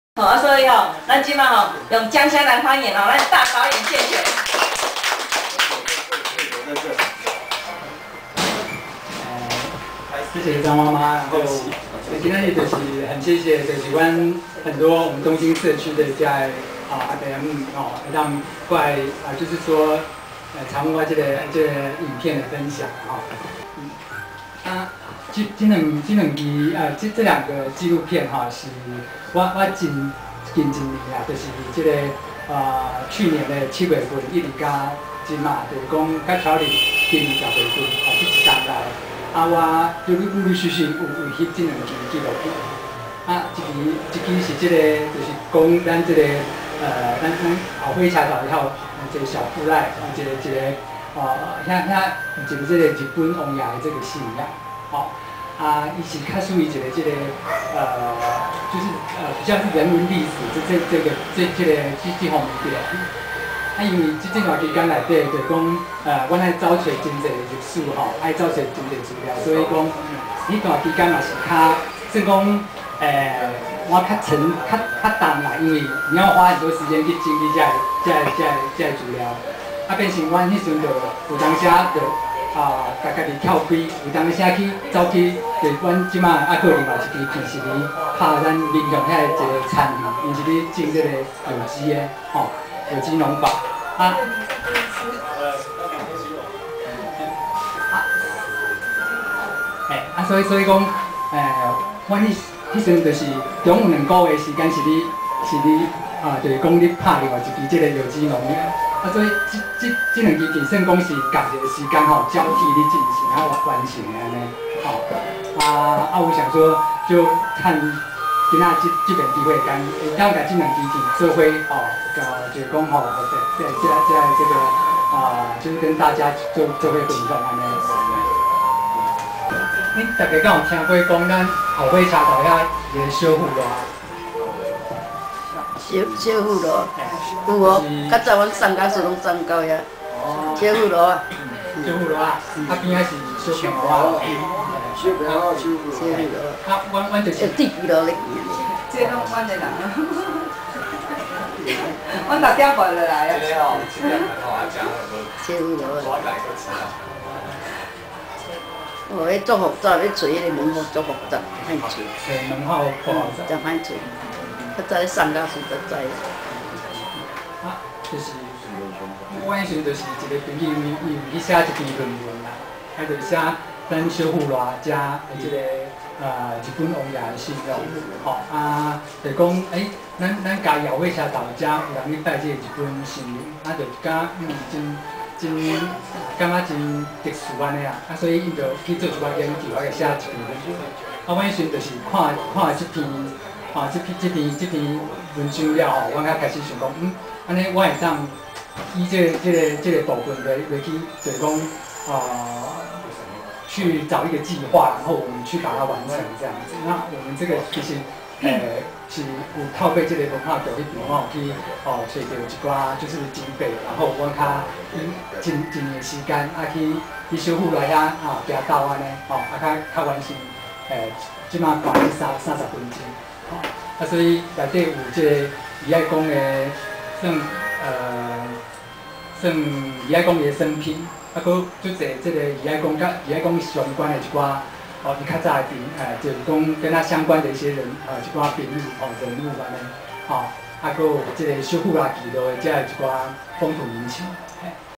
20121227 【漫遊嘉義古聚落-小副瀨的義愛公信仰】與【甦活田園】映後座談(1)
嘉義後火車站附近的「小副瀨」聚落，當地居民為何崇拜信仰日本警察王爺─「義愛公」呢？聽聞民雄西昌社區的「憨己園」裡面不只生態豐富，甚至森林和溼地都看得到，園主是如何找出這麼友善土地的耕作模式呢？